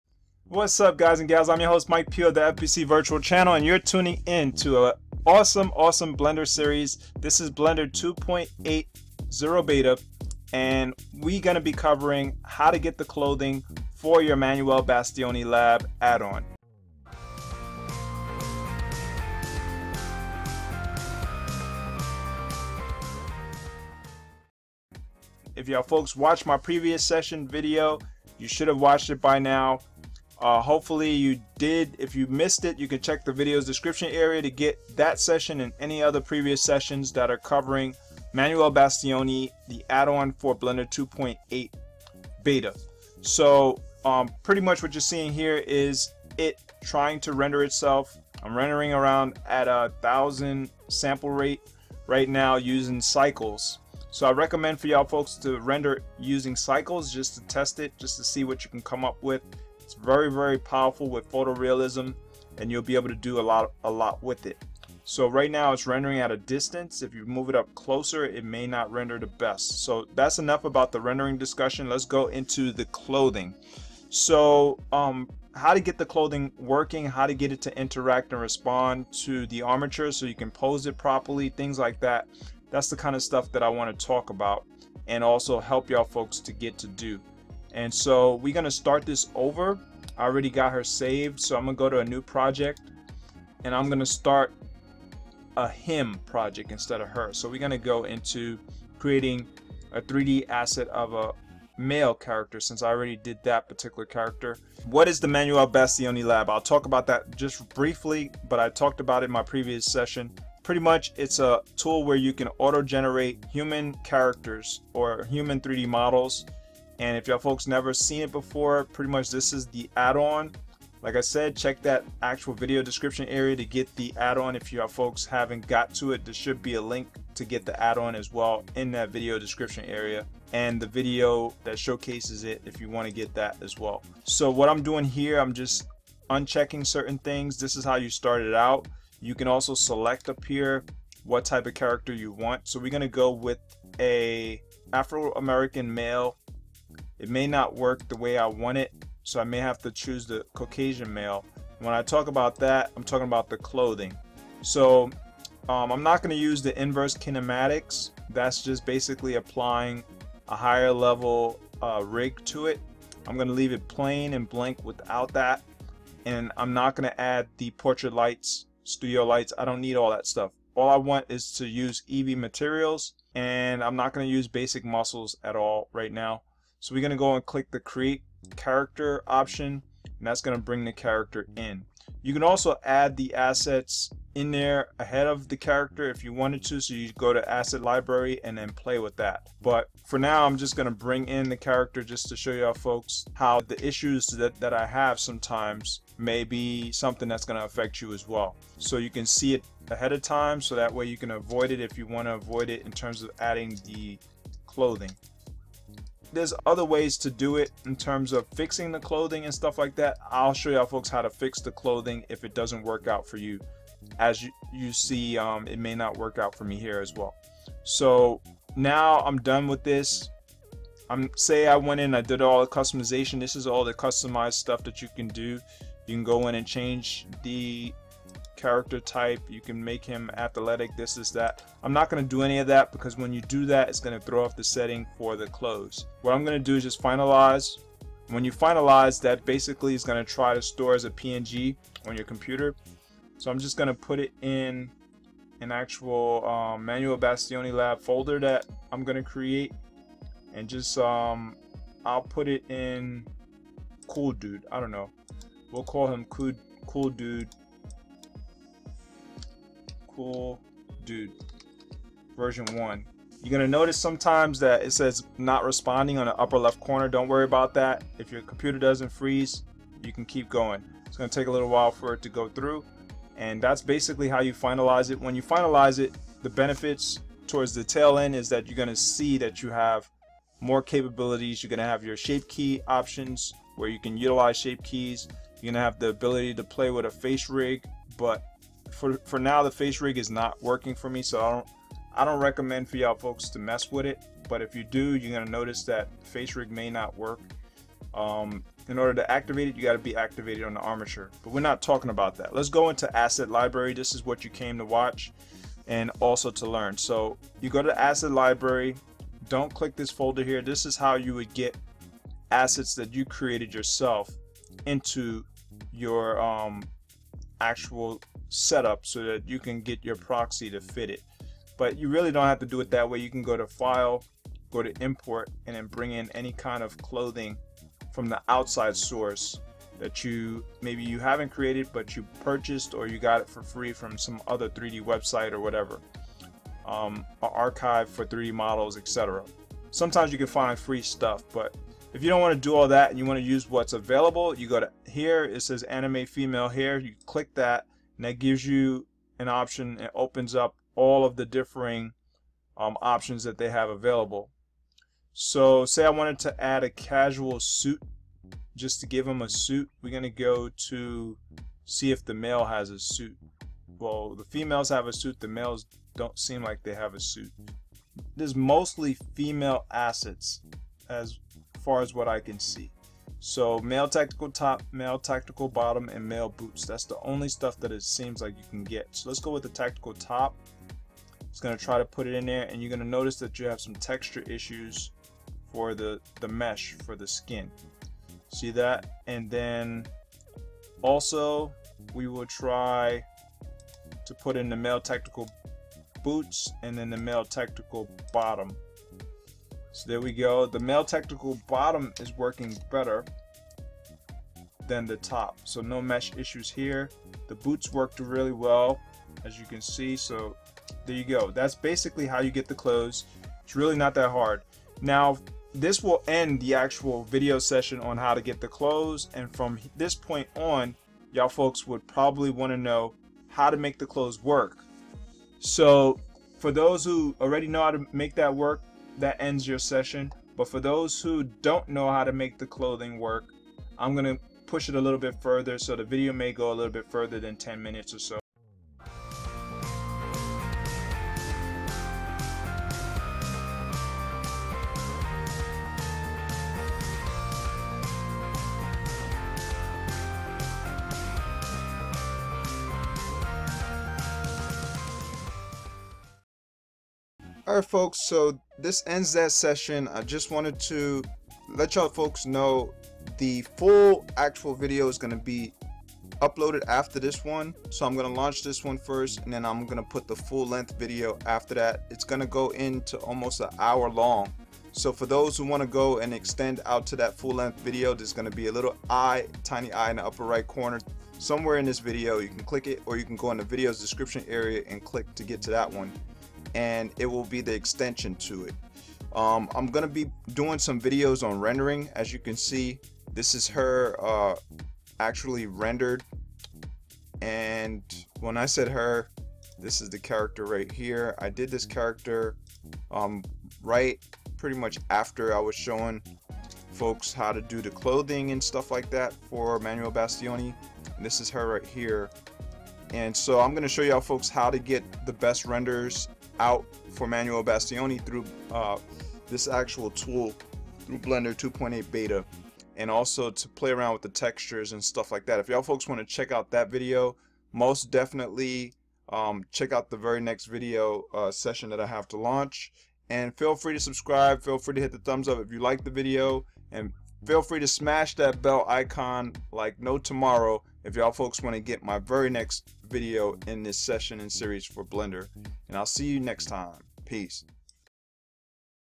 Manuel Bastioni Lab Clothing Assets For Blender 2.8 Beta (Part I Quick How-to)
Manuel Bastioni Lab is still in full effect for Blender 2.8 Beta. In this video you'll learn how to add clothing assets. Looking to get the add-on for your Blender projects link provided below.